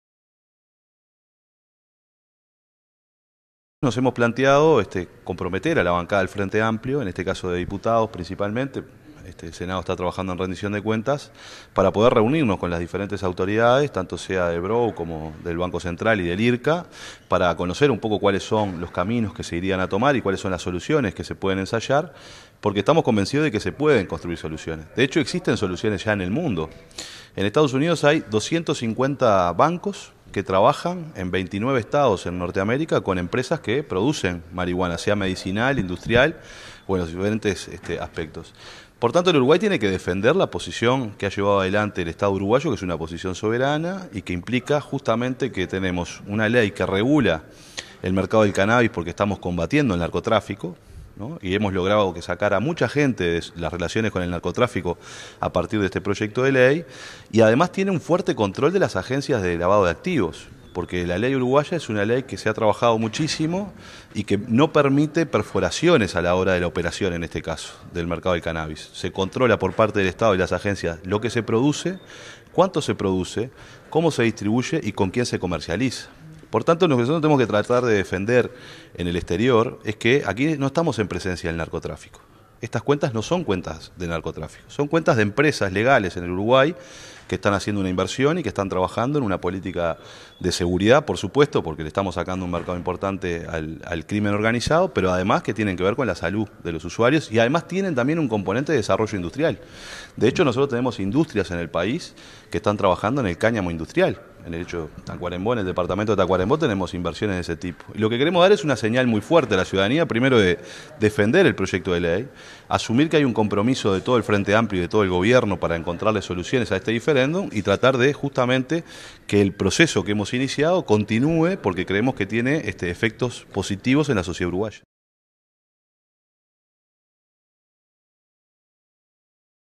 MPP pidió el compromiso de los parlamentarios del FA en defensa de la ley de regulación del cannabis
El diputado Alejandro Sánchez dijo que Uruguay puede pensar en medidas que existen en el mundo para solucionar el problema del sistema financiero.